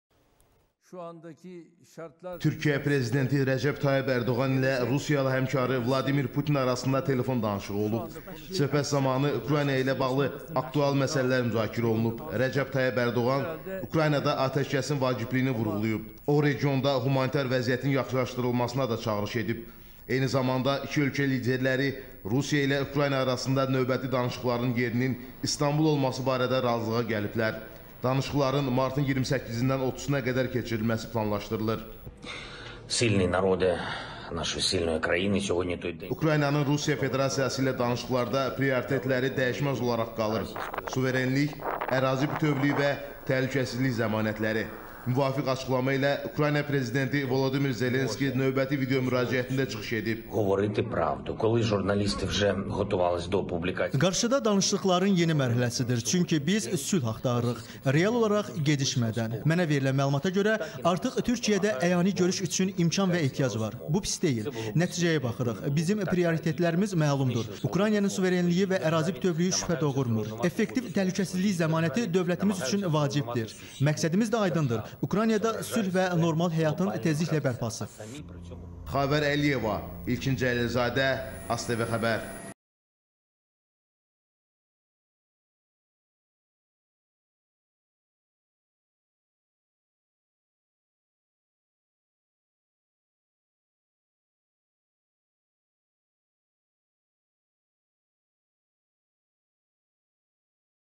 Rusiya-Ukrayna danışıqları İstanbulda keçiriləcək
Ukraynada müharibənin 33-cü günüdür. Rusiya və Ukrayna arasında danışıqların növbəti mərhələsi İstanbulda keçiriləcək. Bu arada, Volodimir Zelenski növbəti dəfə xalqa videomüracət edib.